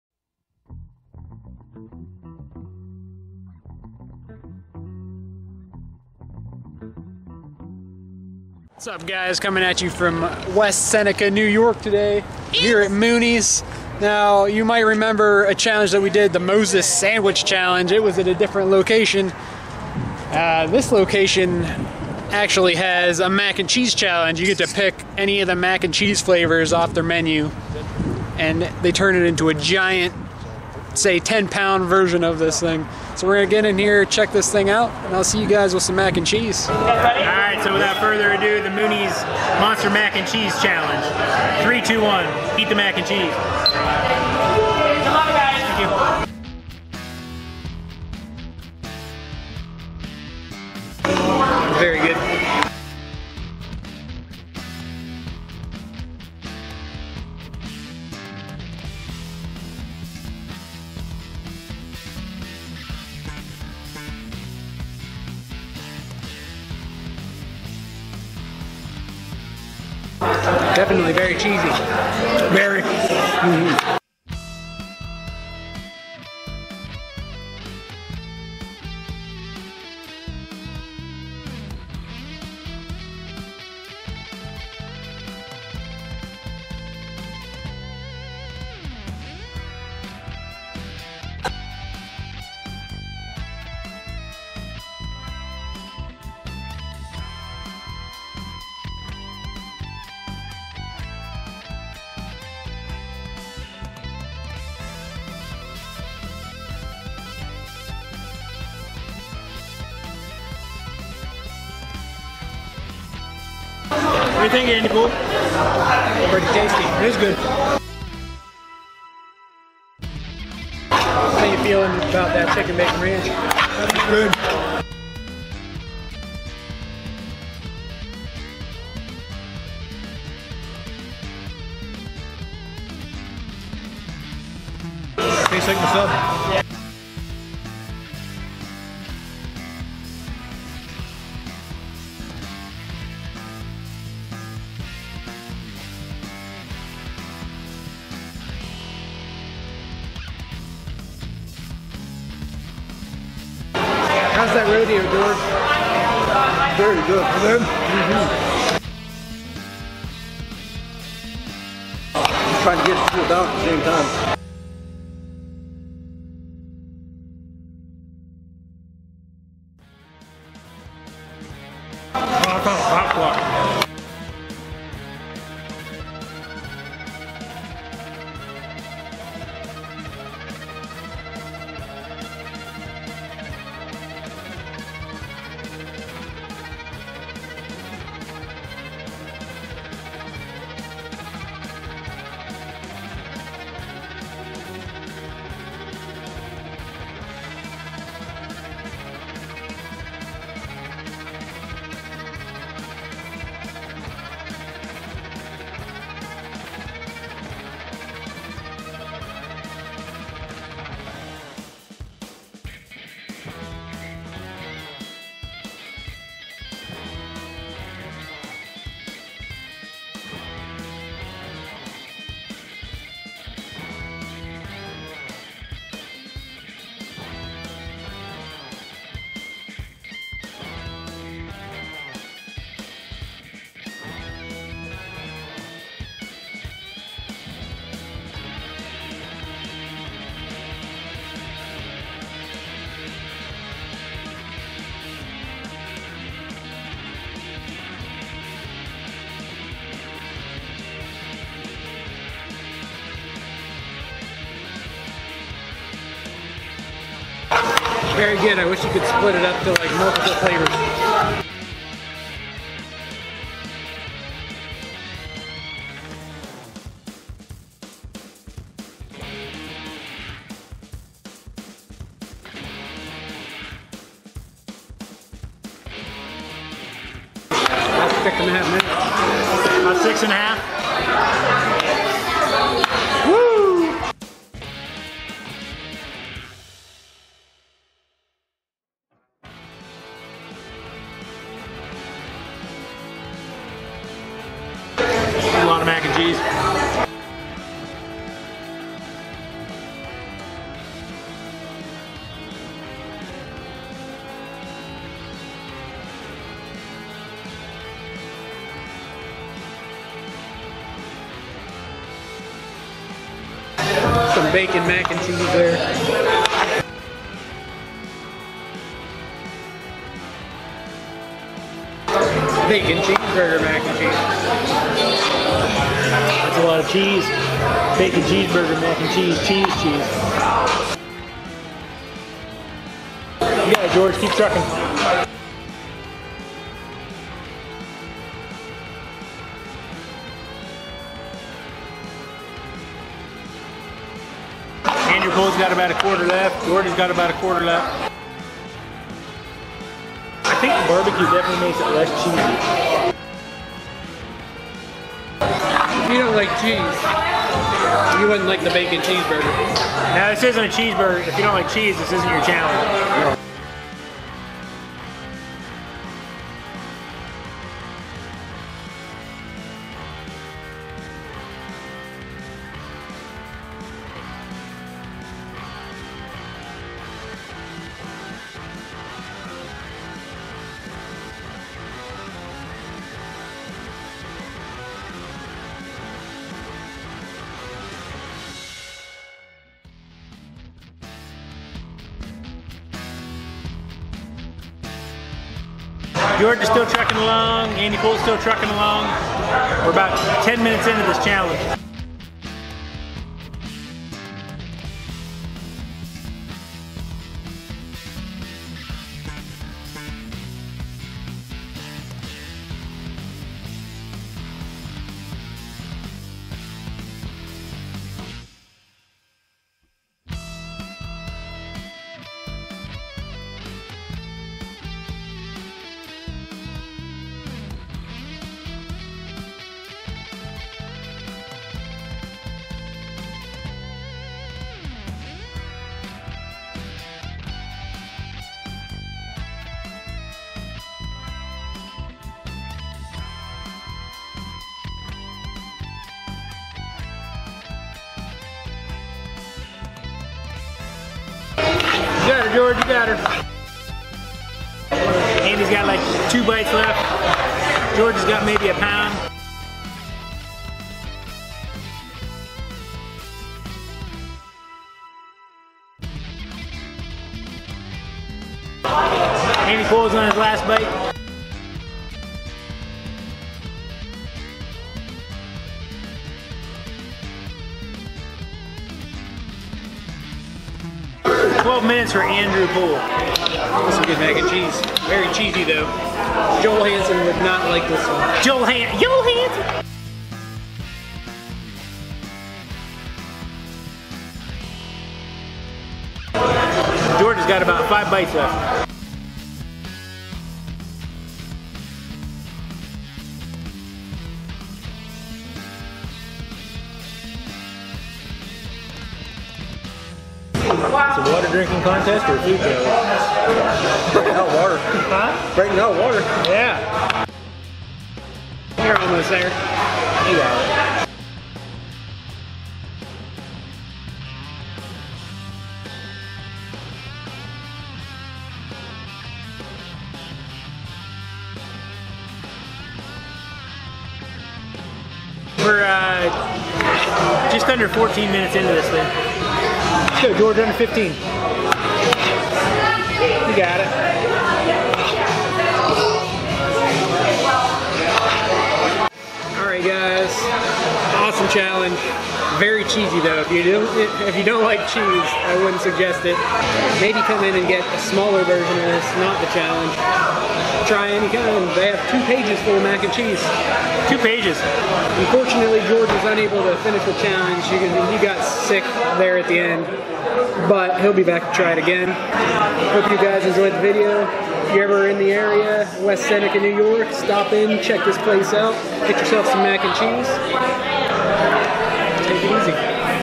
The Monster Mac Challenge
For this weeks video, I'm coming at you from New York with another Restaurant Challenge. Mooney's Sports Bar & Grill is home to an absolutely monstrous macaroni and cheese challenge. It all begins with choosing your favorite mac and cheese off of their 2 full menu pages of mac and cheese. They then cook up an entire skillet of it and bring it to your table. If you are able to consume all of it within 30 minutes, you receive your meal for free and your picture on their wall of fame. Do you think that I have what it takes to defeat this massive creation? You'll have to watch and see!
Mooney's Sports Bar & Grill
DAN KILLER KENNEDY
Monster Mac Challenge
Mac and Cheese